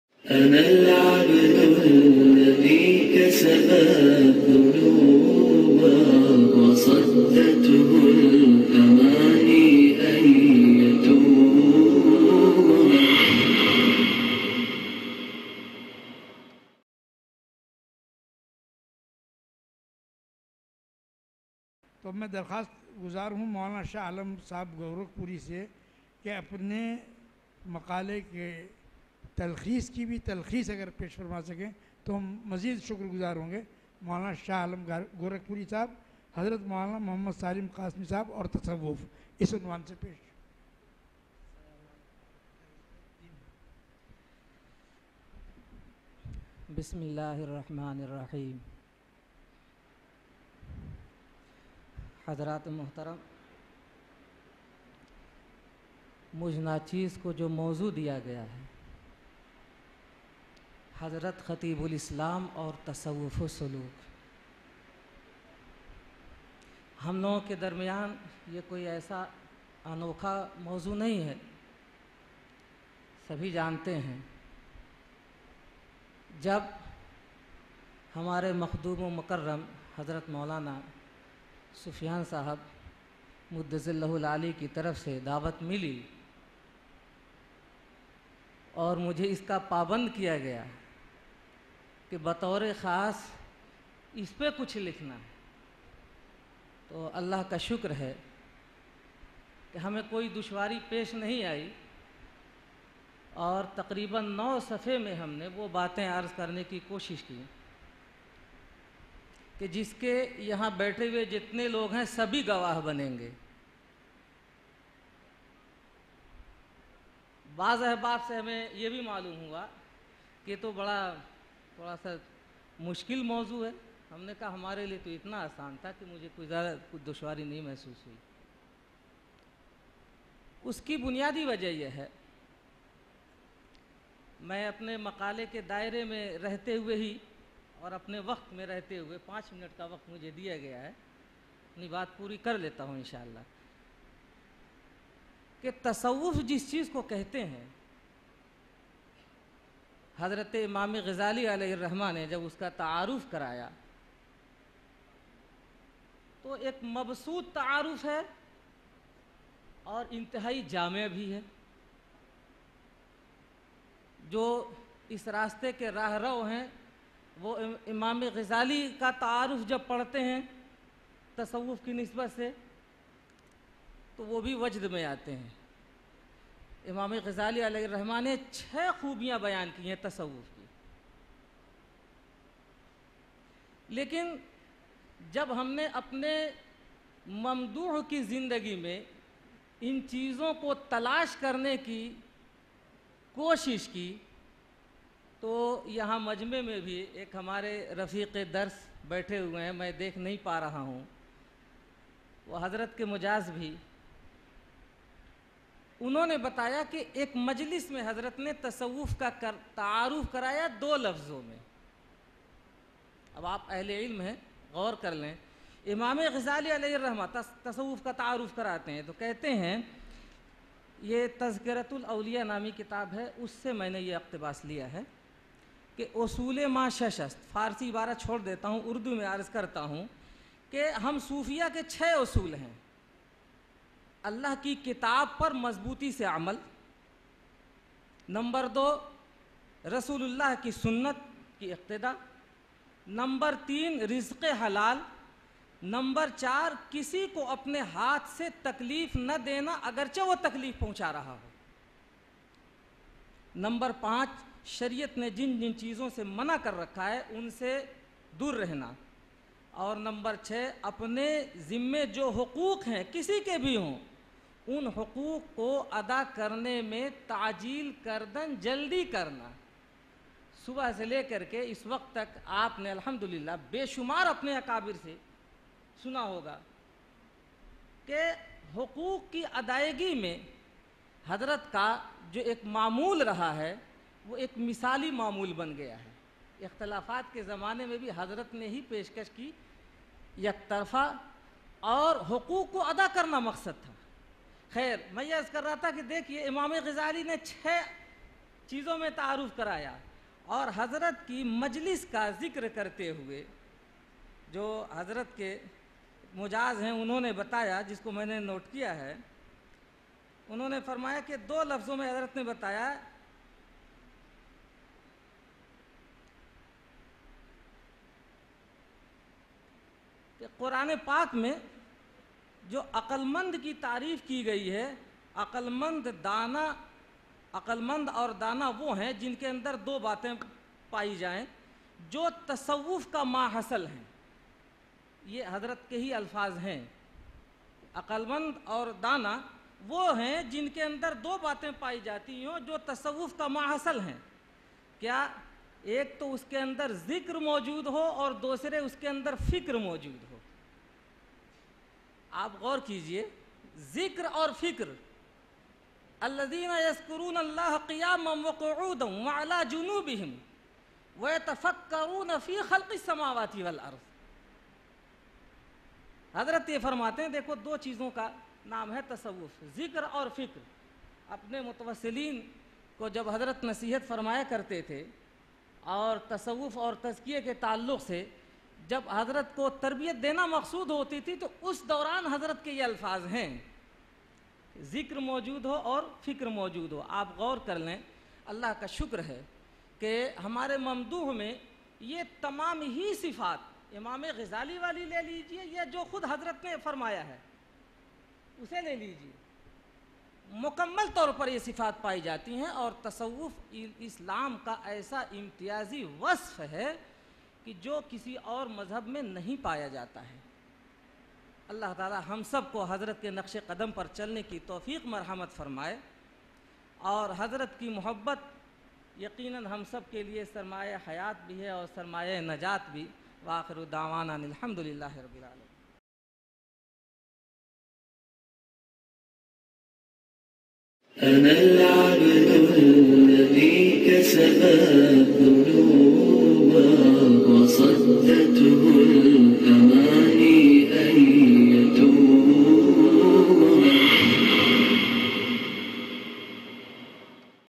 Khateeb ul Islam International Seminar (Paper by: Maulana Shah Alam Gorakhpuri Sb)
بین الاقوامی سیمینار بعنوان “خطیب الاسلام حضرت مولانا محمد سالم قاسمی صاحب رحمہ اللہ: حیات وکارنامے” منعقدہ ۱۲-۱۳ اگست ۲۰۱۸م۔
زیر اہتمام: حجۃ الاسلام اکیڈمی، دارالعلوم وقف دیوبند

مقالہ نگار: مولانا شاہ عالم صاحب گورکھپوری (ناظم شعبۂ تحفظ ختم نبوت، دارالعلوم دیوبند)

International Seminar on Life and Achievements of Khateeb ul Islam Hazrat Maulana Mohammad Salim Qasmi (RH) organised by Hujjat al Islam Academy, Darul Uloom Waqf Deoband, Dated 12-13 Aug 2018.

Paper Presented by: Maulana Shah Alam Sb Gorakhpuri (Dean, Tahaffuz e Khatm e Nubuwwat, Darul Uloom Deoband)